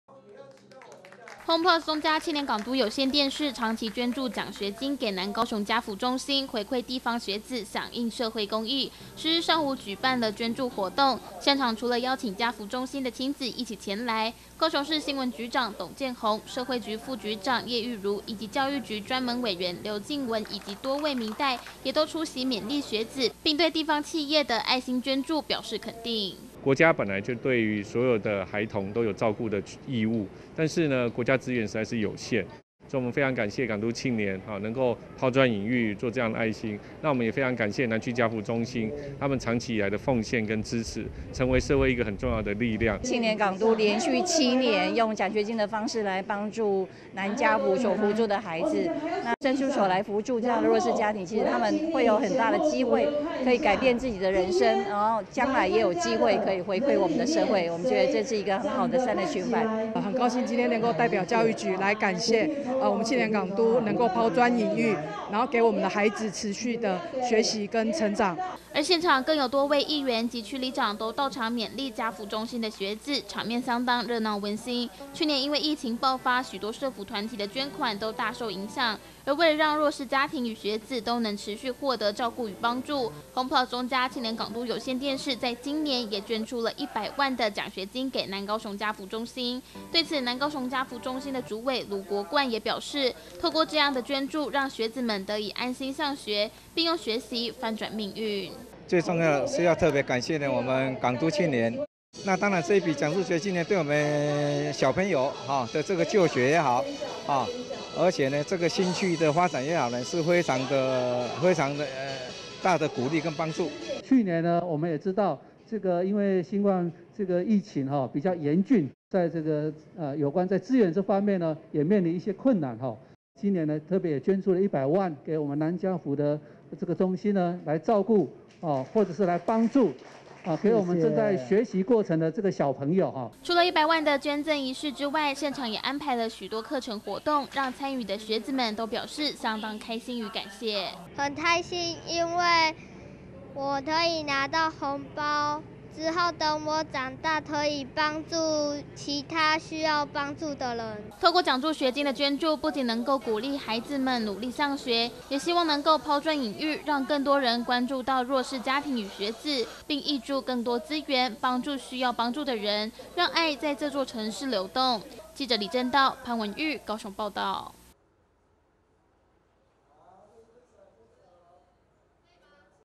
愛心公益 | 慶聯港都響應公益 捐助家扶百萬獎學金
慶聯港都響應公益 捐助家扶百萬獎學金

HOME+中嘉慶聯港都有線電視長期關心弱勢，連續第七年捐助獎學金給南高雄家扶中心，希望能夠回饋地方，照顧弱勢學子。10日上午舉辦了捐助獎學金活動，高雄市新聞局長董建宏、社會局副局長葉玉如以及教育局專門委員和多位民意代表都出席肯定，現場更安排許多課程與活動讓家扶中心的孩子參與，現場氣氛熱鬧溫馨。